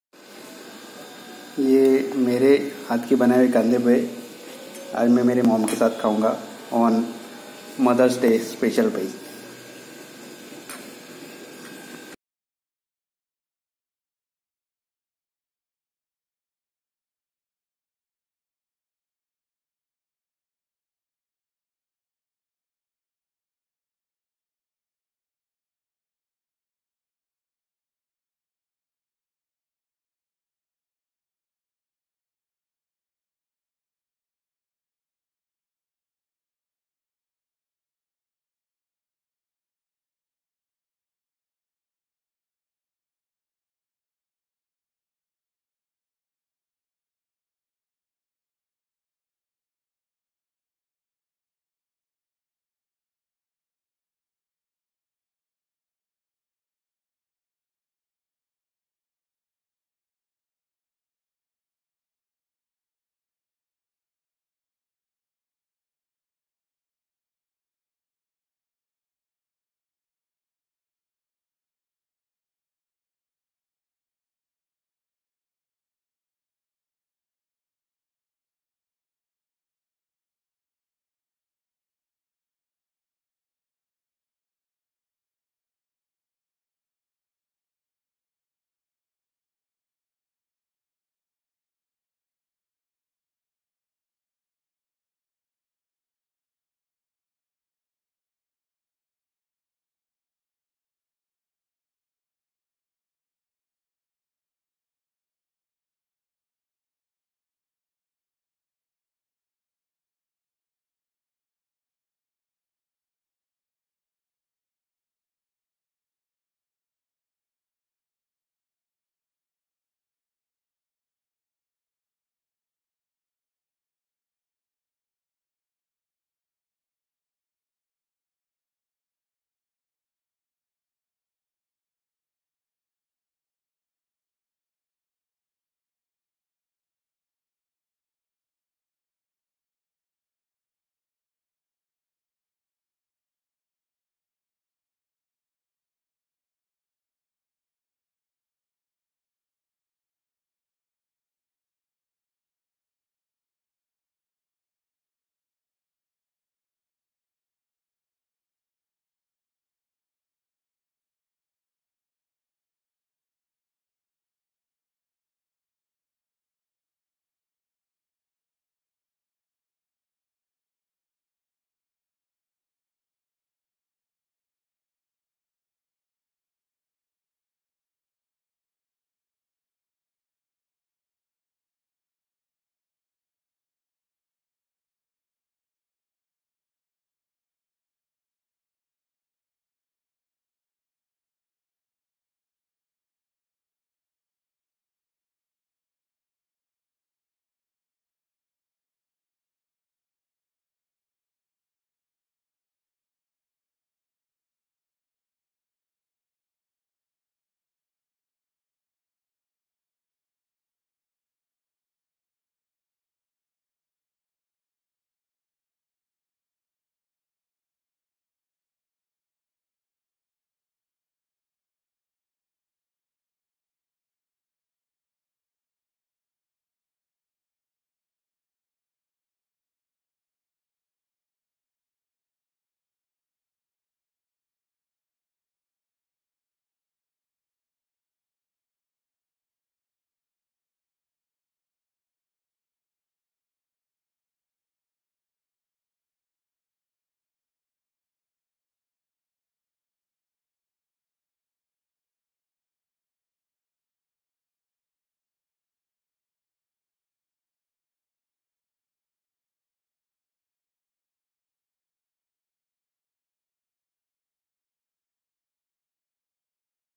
DIY - Mother's day special recipe - Kande Pohe
Made kande pohe for me and mom

Ingredients
2 small size onions
1 tble spoon mustard seeds
1 table spoon cumins seeds
1/2 cup ground nuts
2 cup  poha (soaked in water)
1/2 cup corriender leaves
1/2 tea spoon turmeric powder